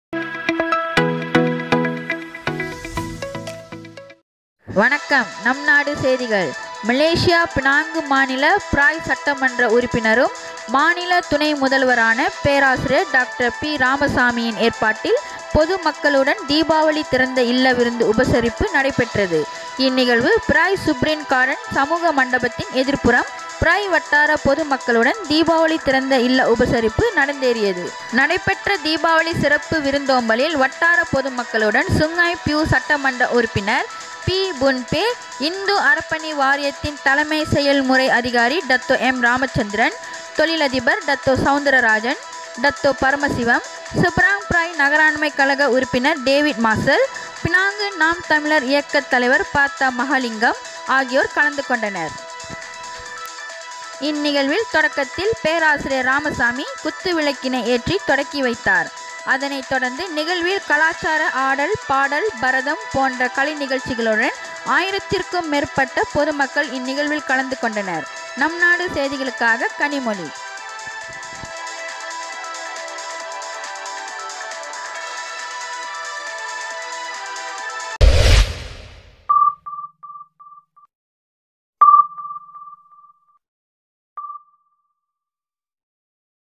NAMNADU TV | PROF DR.RAMASAMY | தீபாவளி திறந்த இல்ல விருந்து உபசரிப்பு
பிறை சட்டமன்ற உறுப்பினரும் மாநில துணை முதல்வரான பேராசிரியர் டாக்டர் பி.ராமசாமியின் ஏற்பாட்டில் பொதுமக்களுடன் தீபாவளி திறந்த இல்ல விருந்து உபசரிப்பு நடைபெற்றது. இன் நிகழ்வு பிறை சுப்ரீன் கார்டன் சமூக மண்டபத்தின் எதிர்புறம் பிறை வட்டார  பொதுமக்களுடன் தீபாவளி திறந்த இல்ல உபசரிப்பு நடந்தேறியது.

       நடைபெற்ற தீபாவளி சிறப்பு விருந்தோம்பலில் வட்டார பொதுமக்களுடன் சுங்கை பியூ சட்டமன்ற உறுப்பினர் பீ பூன் பே, இந்து அறப்பணிவாரியத்தின் தலைமை செயல்முறை அதிகாரி டத்தோ எம்.இராமசந்திரன், தொழிலதிபர் டத்தோ. செளந்தரராஜன், டத்தோ பரமசிவம், செபராங் பிறை நகராண்மை கழக உறுப்பினர் டேவீட் மார்சல் பினாங்கு நாம் தமிழர் இயக்கத் தலைவர் ப.த.மகாலிங்கம் ஆகியோர் கலந்துகொண்டனர்.

        இன் நிகழ்வில் தொடக்கத்தில் பேராசியர் இராமசாமி  குத்துவிளக்கினை ஏற்றி தொடங்கி வைத்தார்.அதனை தொடர்ந்து நிகழ்வில் கலாச்சார ஆடல்,பாடல்,பரதம் போன்ற கலைநிகழ்சிகளுடன் 1000 க்கும் மேற்பட்ட பொதுமக்கள் இன்நிகழ்வில் கலந்து கொண்டனர்.